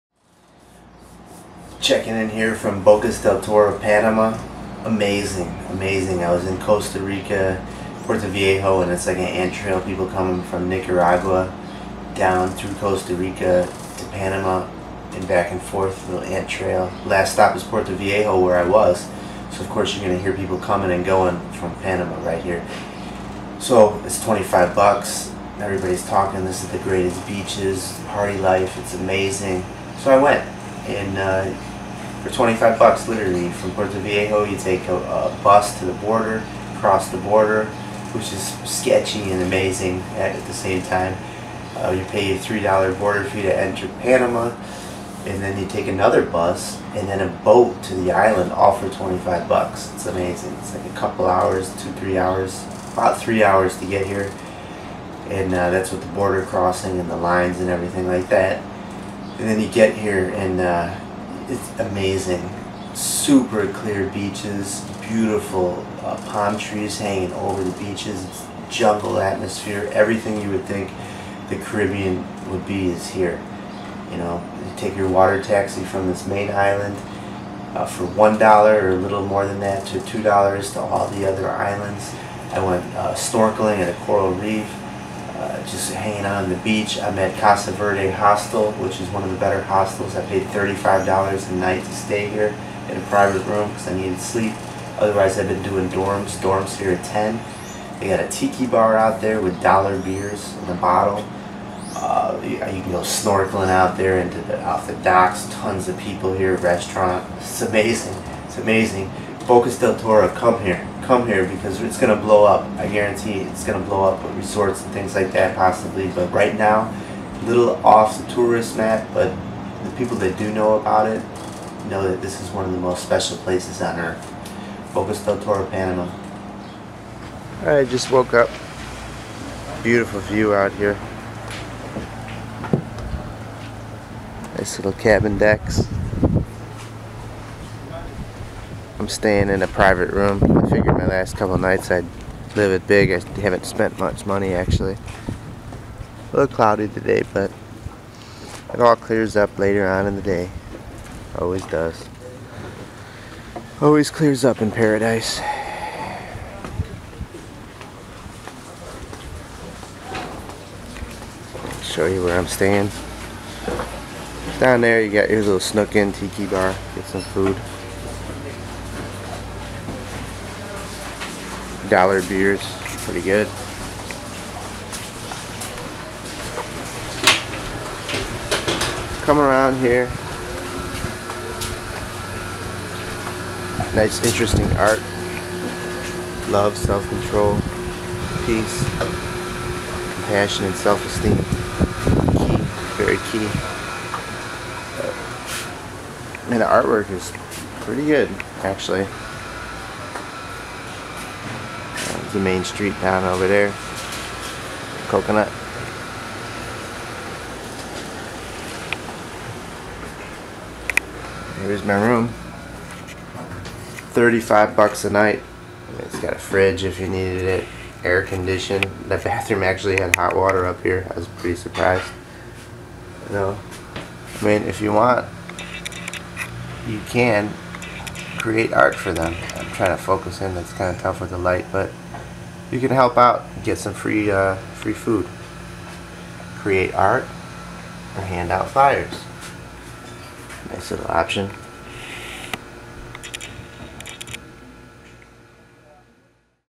Bocas Del Toro, Panama (Casa Verde Hostel)
I explain to you about my experience in Bocas Del Toro, and tell a bit about the hostel I stayed at called Casa Verde.